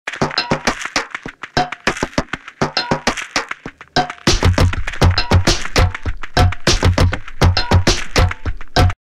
LewisOne Brain Head Turn Pencil Test
Decided to reminisce and do a lunch project between other animated projects in our studio.  This is where it all started for me.